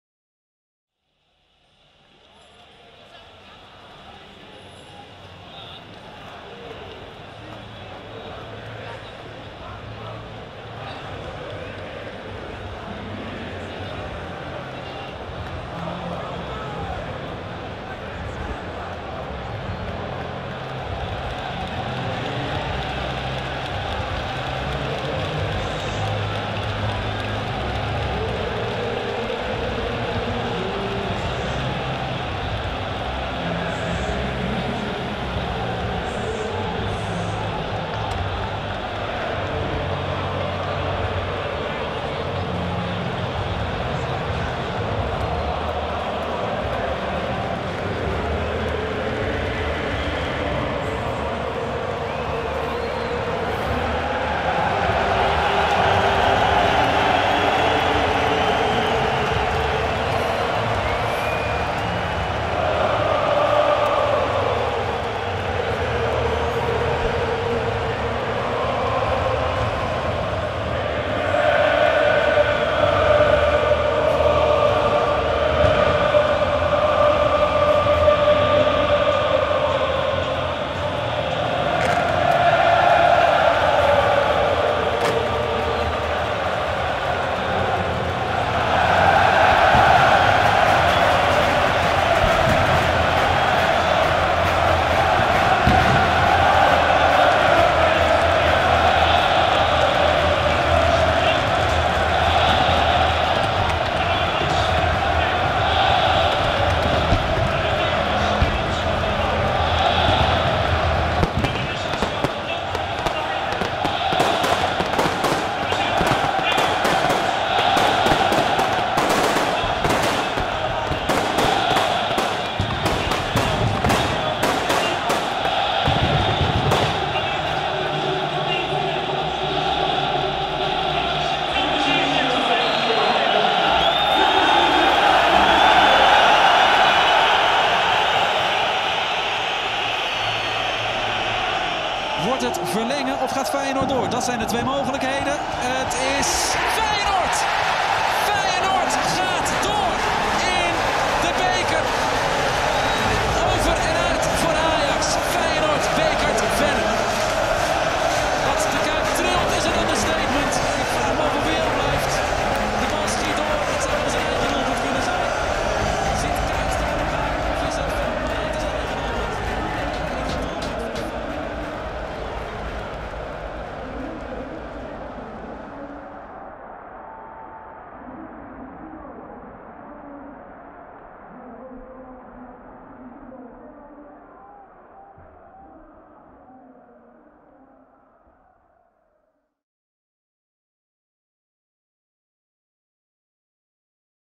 360 video - The Feyenoord-Ajax Derby by Scopic
During the classic derby between Feyenoord Rotterdam and Ajax Amsterdam, Scopic dove straight into the action and shot ‘de Kuip’ stadium in 360 degrees. Needless to say, the results were prodigious in terms of originality and atmosphere; shooting spherical video content in combination with immersive sound engineering is unique in the sporting world - and the technique lends itself perfectly!

From these unique vantage points in the stadium, the viewer can witness events that can normally only be experienced by his or her physical presence. From the revelation of the humongous banner on the north side of the stadium, to the players entering the pitch for the first time: Scopic offers you the best seat in the house.